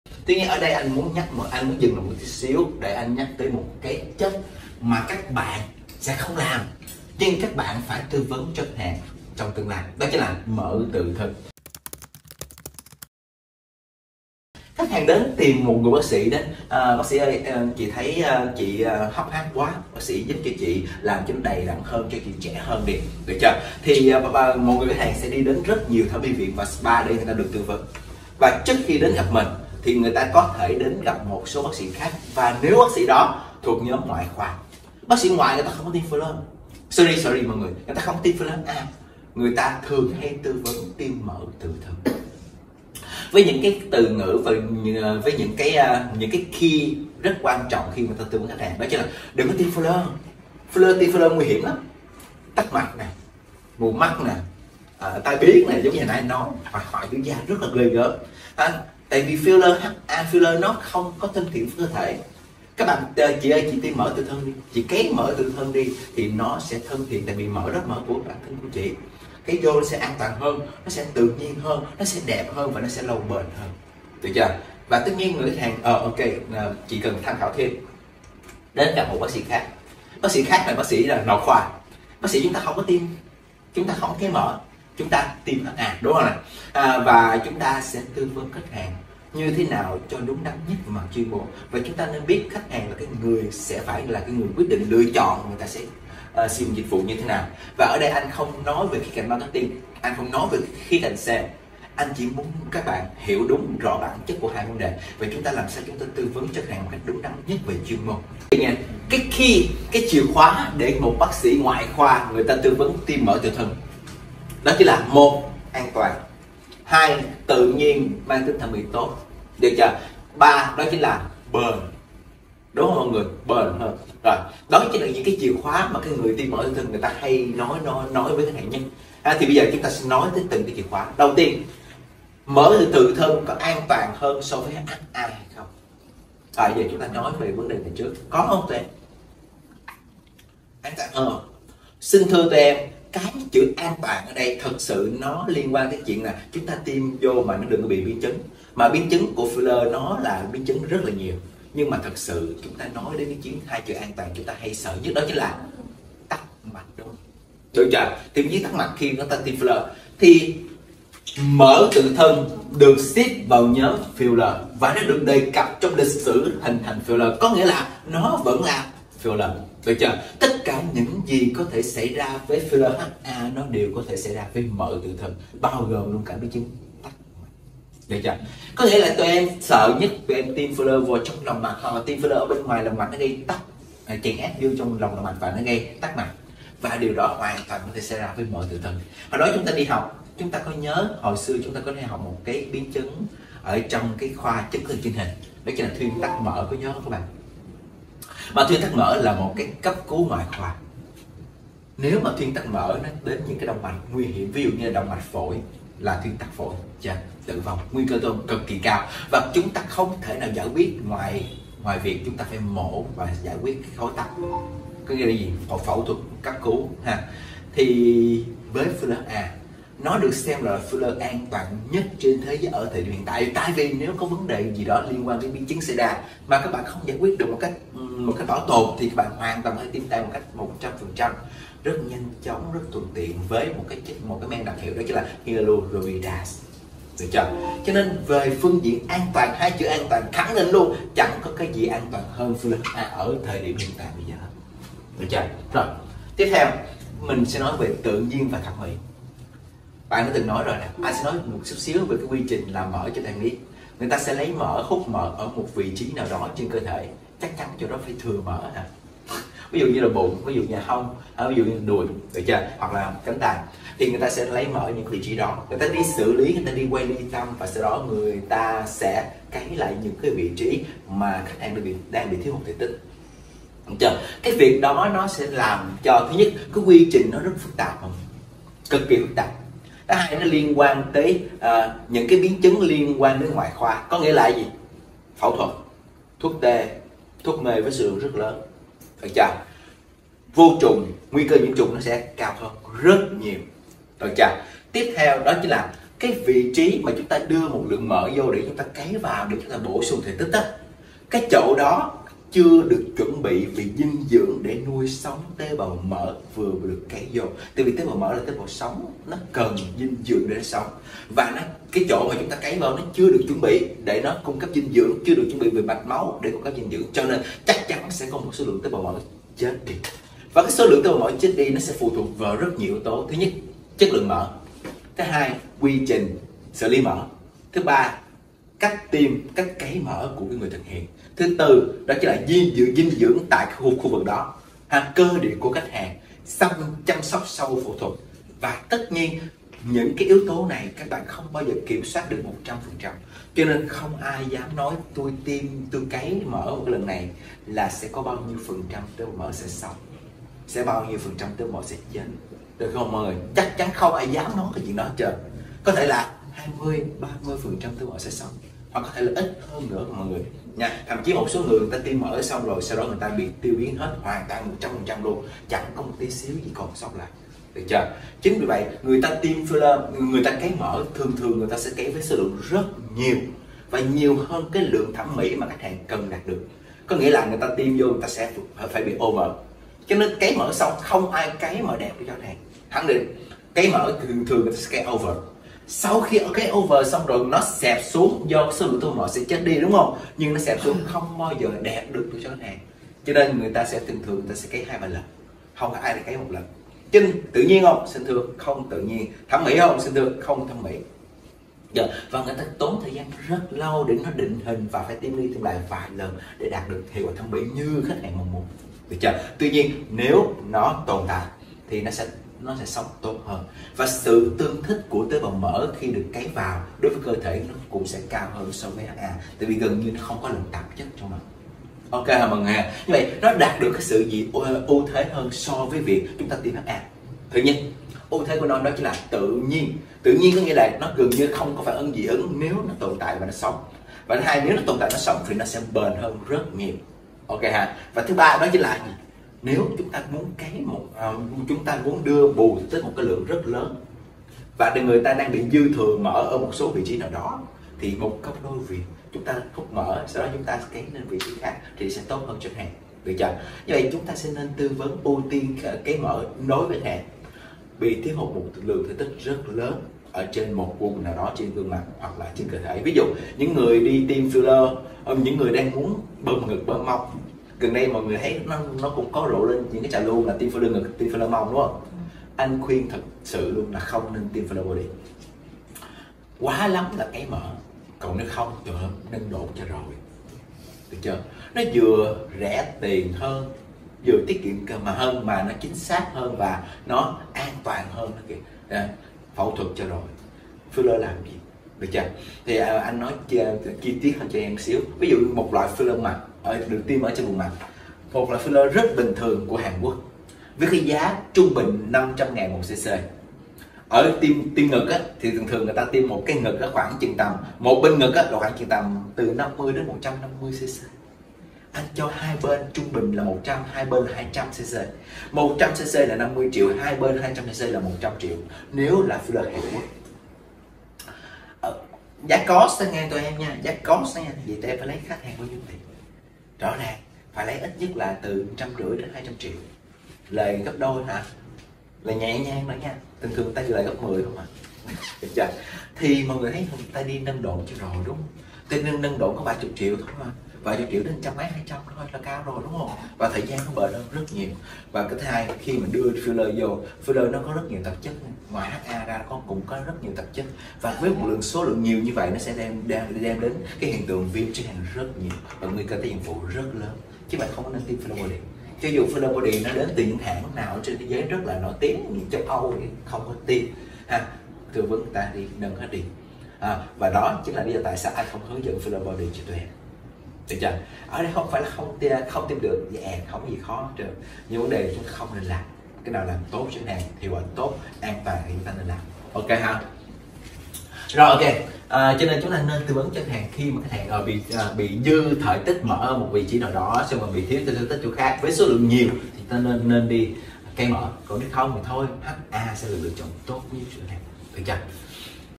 CẤY MỠ TỰ THÂN - Hiểu trước khi thực hiện - Bs Chánh HABOS ACADEMY
Bạn được tư vấn tiêm mỡ tự thân sao?
Hãy nghe bác sĩ Chánh giải thích trước khi quyết định nhé 😊